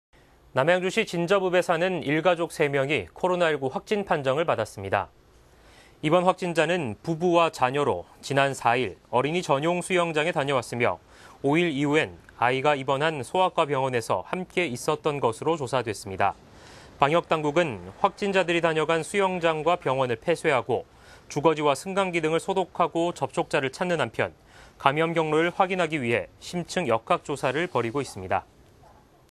진접읍 거주 일가족 3명 코로나19 확진(서울경기케이블TV뉴스)
【 앵커멘트 】
남양주시는 지난 11일
진접읍에 살고 있는
30대 여성이 코로나19 확진 판정을
받았다고 밝혔습니다.

【 VCR 】

해당 확진자는 지난 6일부터
발열과 기침 등 증상이 나타났으며,
10일 선별진료소를 찾아
진단검사를 받은 뒤
같은 날 확진판정 받았습니다.

이후 가족들에 대한 검체채취가
진행됐고, 남편과 자녀 한 명을
포함해 일가족 모두
코로나19에 감염된 것으로 확인됐습니다.

빙역당국은 이들이 다녀간
수영장과 병원을 폐쇄하고,
주거지 등을 중심으로
접촉자 파악에 나서고 있습니다.